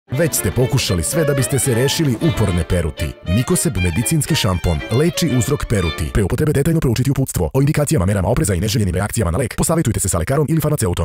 Mycoseb (11s)
Mycoseb medicinski šampon, leči uzrok peruti.

Mycoseb medicinski šampon deluje na gljivice i time leči perut i seboroični dermatitis kosmatog dela glave.
Ipak je to Hemofarm.